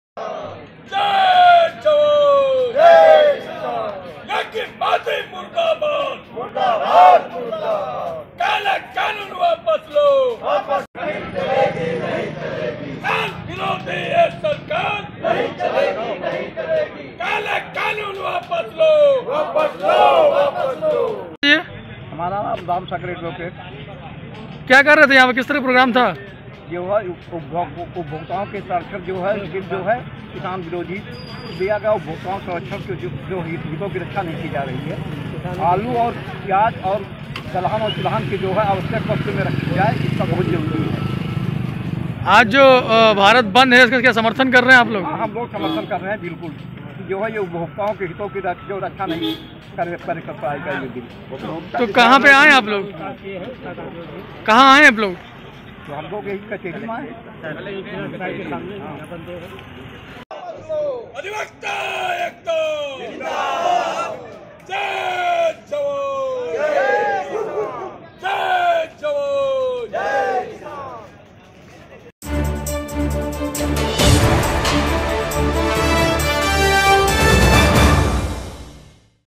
किसानों के समर्थन में वकीलों ने किया प्रदर्शन
रायबरेली में कलेक्ट्रेट के वकीलों ने किया प्रदर्शन, किसानों के समर्थन में वकीलों ने किया प्रदर्शन। जिलाधिकारी को सौंपा ज्ञापन।  केंद्र सरकार से किसानों की समस्याओं को जल्द से जल्द समस्या समझाने की कही बात।